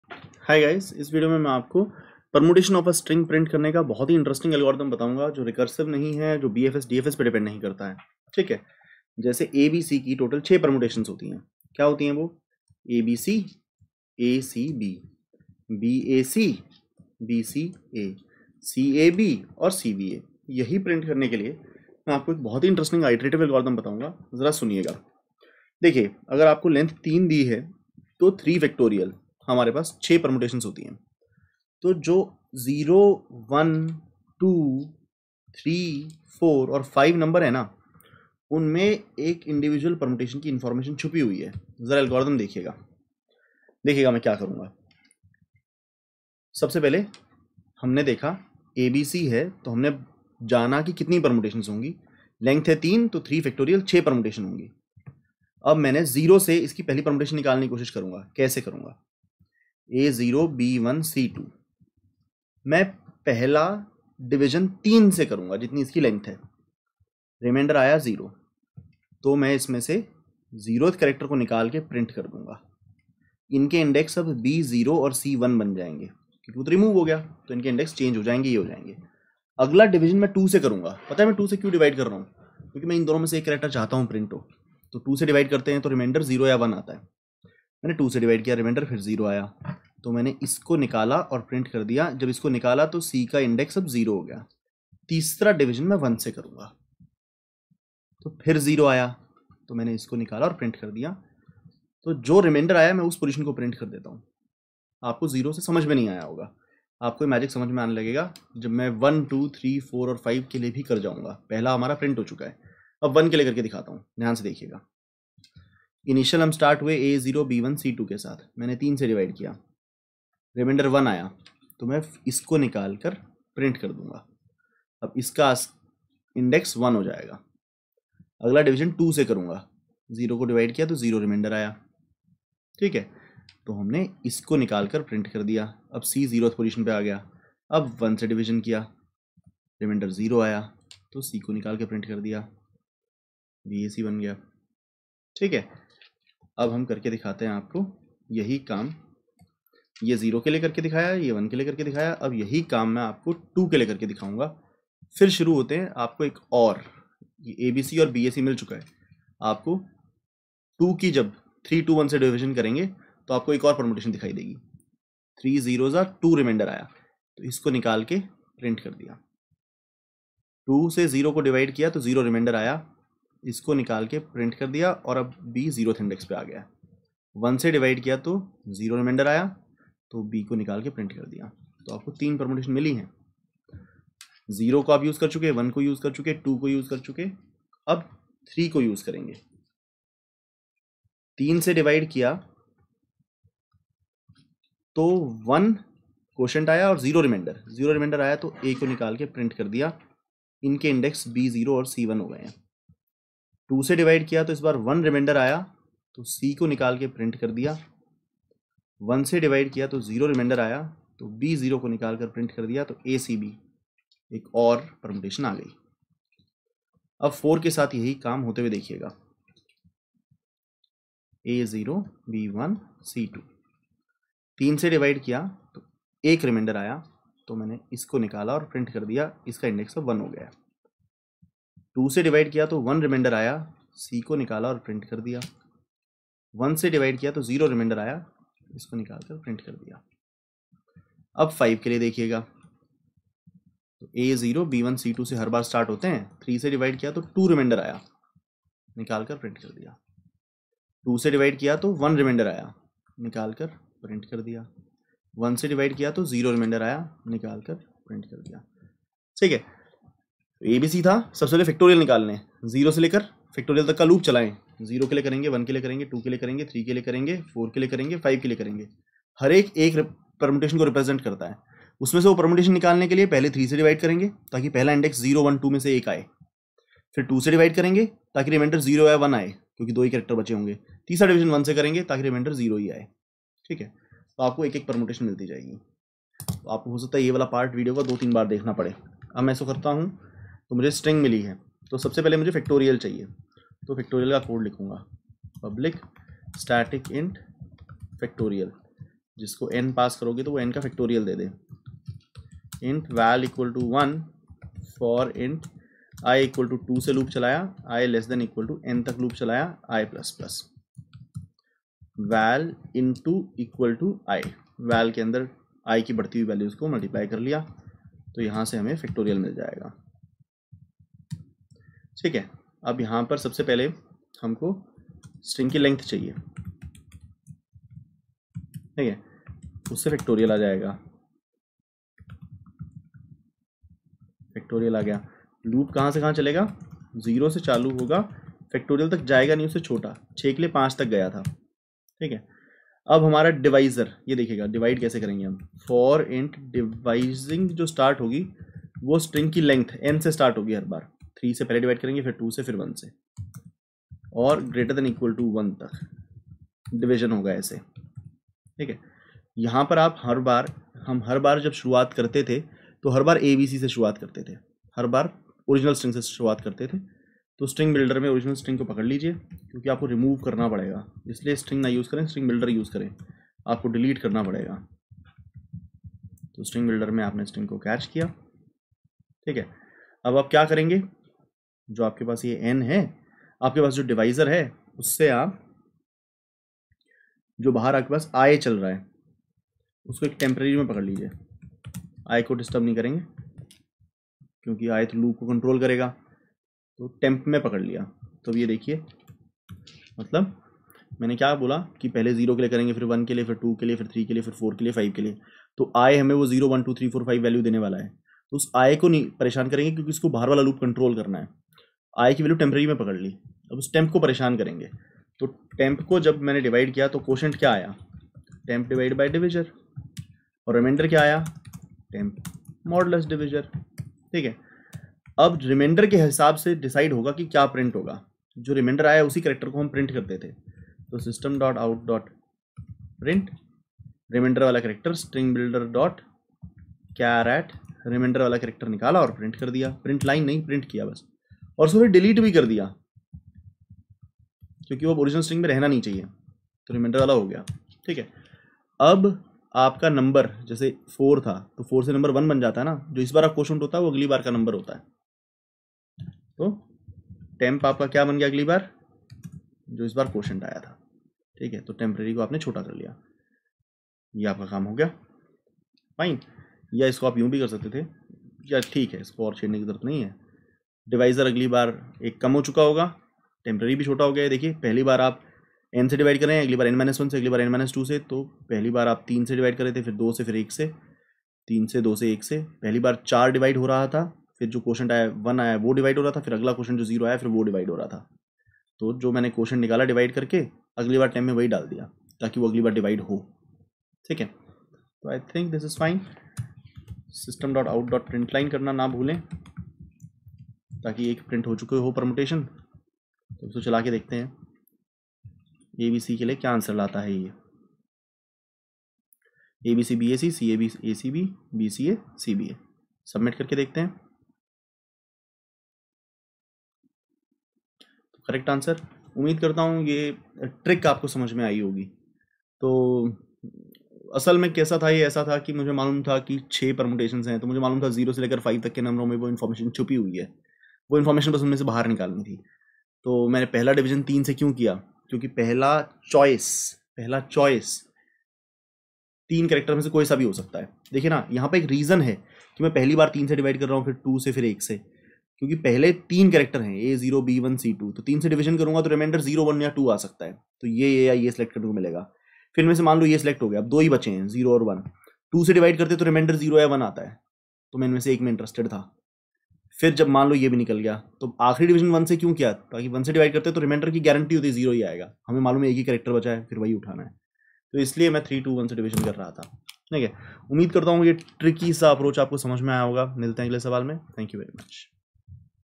Permutations of a String - Iterative Solution | String and Stringbuilders in JAVA
NADOS also enables doubt support, career opportunities and contests besides free of charge content for learning. In this video, we discuss the Permutations of a String problem with an iterative solution using the concepts of strings and stringbuilder. In this problem,

1. You are given a string.
2. You have to print all permutations of the given string iteratively.